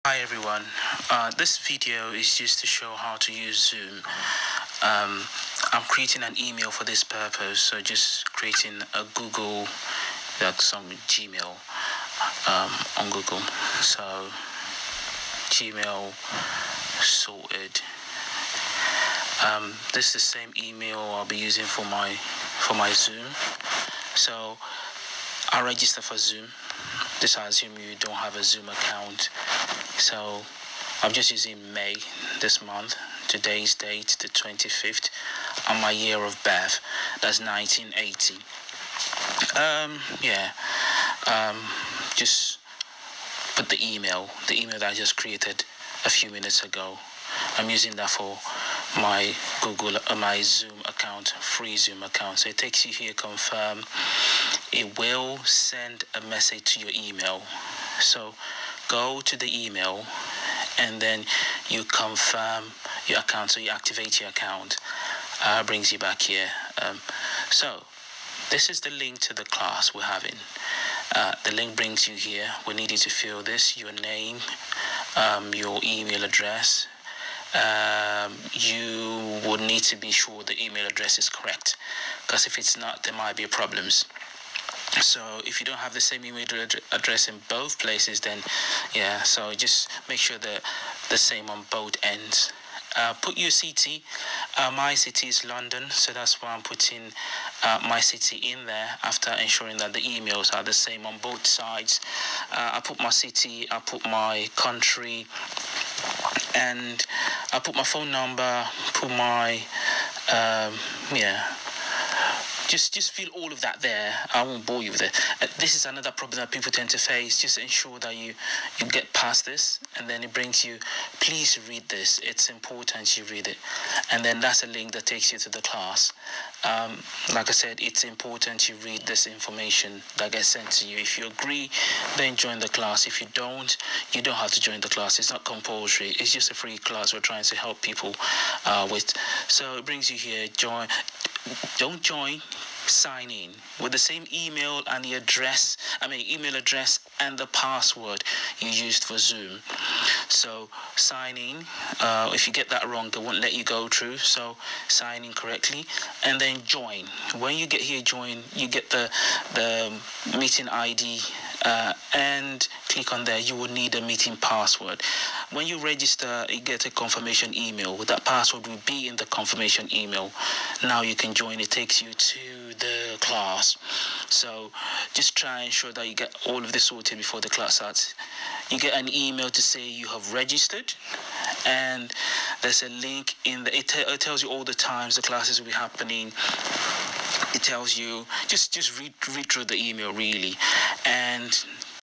How to join our FREE IELTS class via Zoom
We aim to make Free IELTS classes available to everyone.

Our  FREE IELTS classes are now on Thursdays 8pm to 10pm Nigerian time

 The tutor will answer any of your IELTS academic questions in real time

If you do not want to turn on your video don’t bother joining our zoom class because you will get kicked out. You can join via YouTube.

For updates 👇🏿

Kind regards